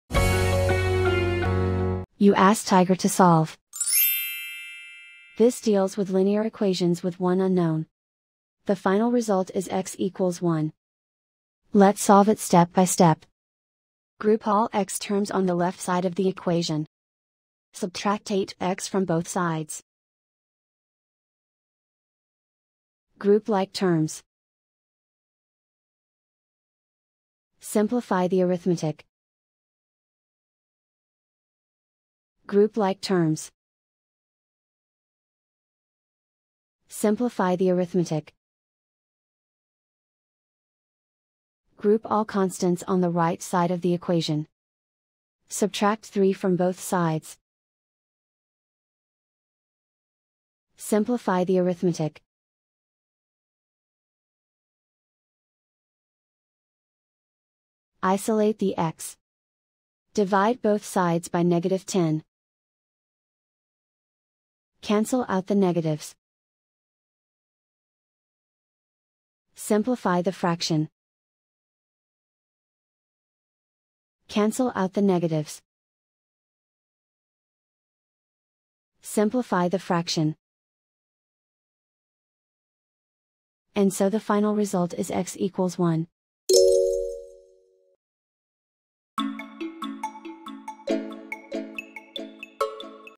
Linear equation with one unknown: Solve -2x+3=8x-7 step-by-step solution
for a step-by-step and more tips and tricks for dealing with linear equations with one unknown.